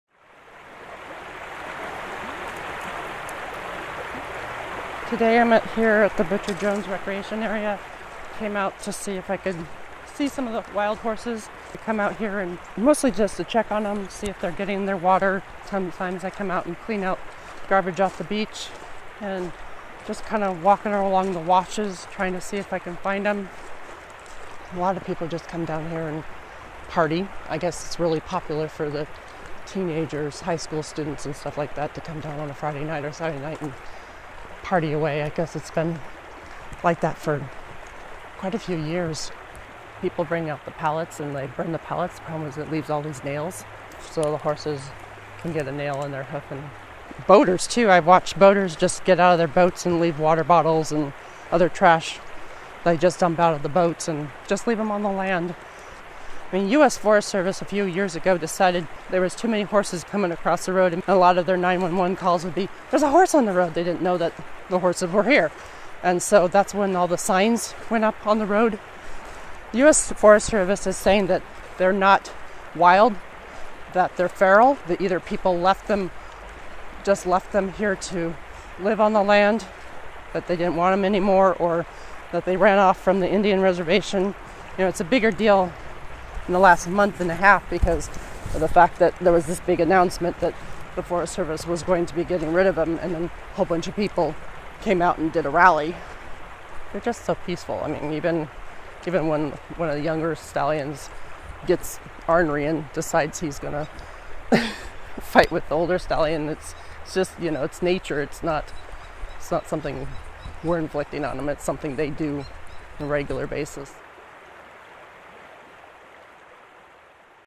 Salt River Horses Photo Slideshow
Janie Neil shares her experiences with the Salt River horses and the Salt River recreation areas.